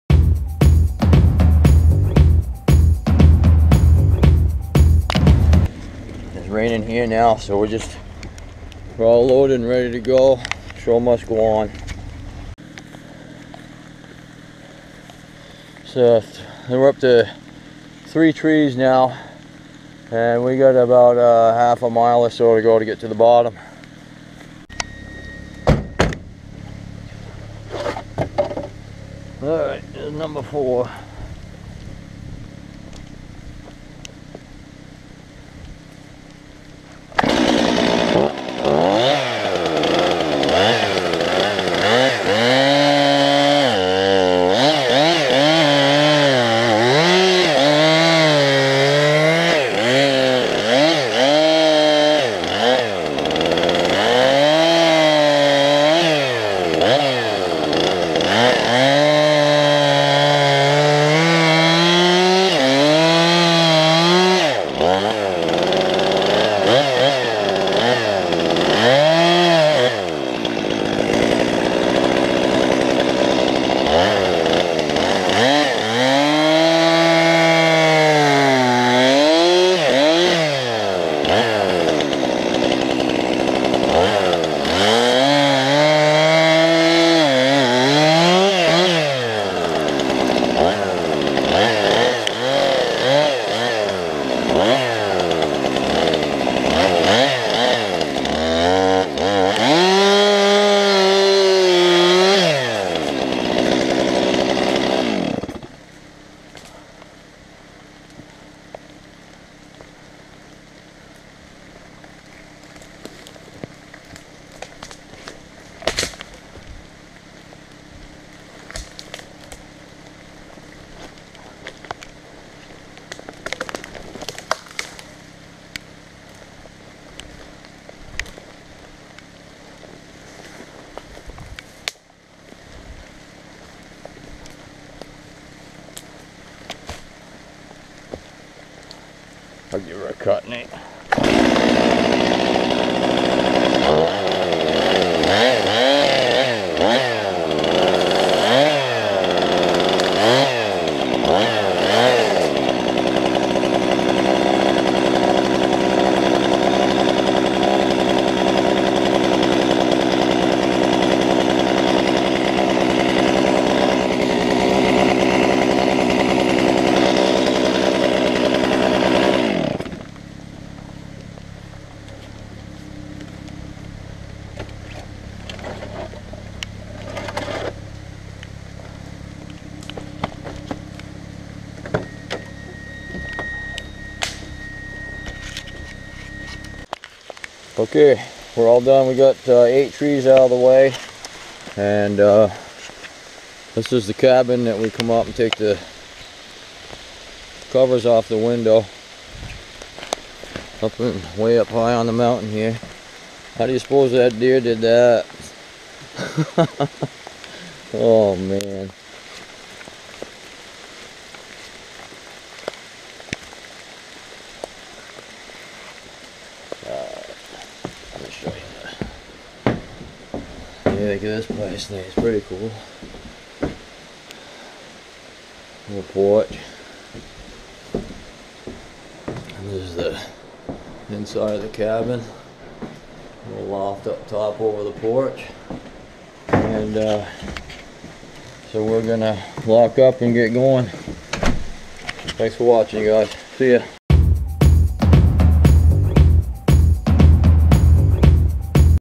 Sthil MS-362 - Trail Clearing With The Subaru Sambar
Take a look at the Sthil MS-362 chainsaw. We'll be clearing the trail up to the small cabin at the top today.